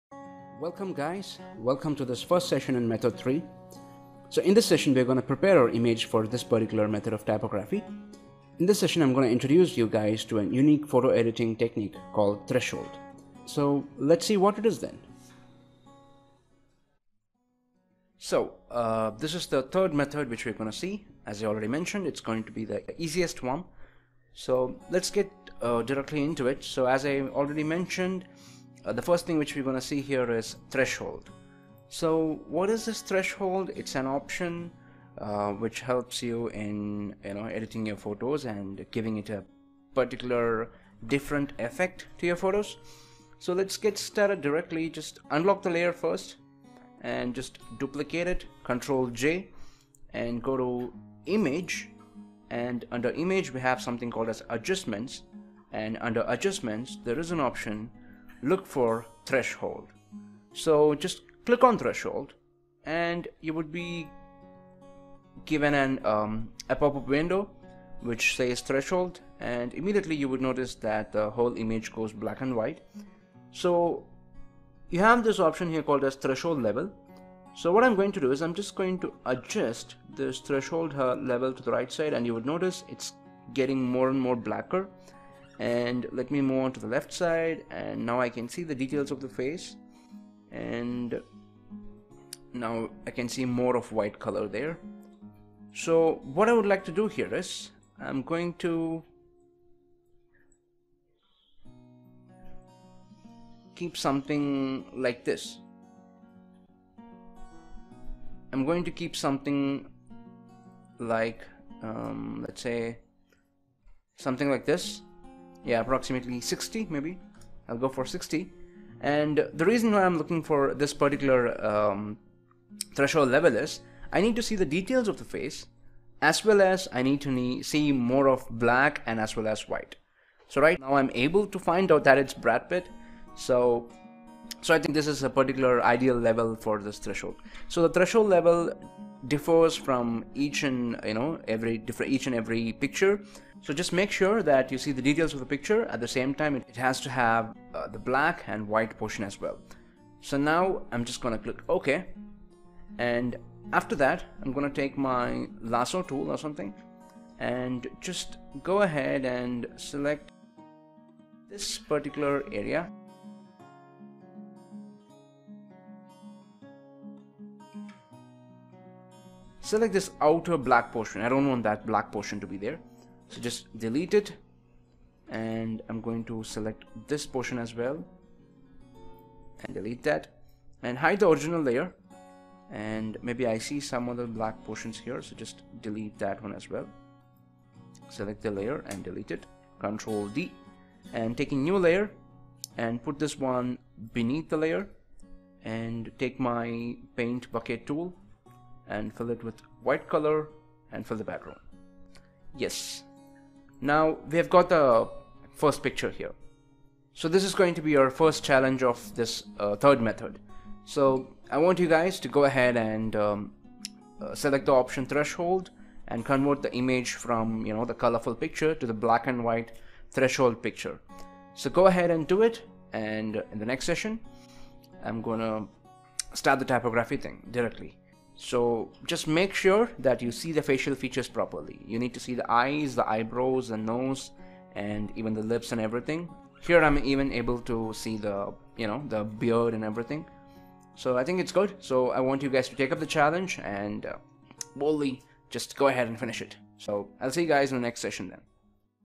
Introduction to Threshold Preparing Image
Learn how to Create Portrait Typography from any picture in less than 3 hours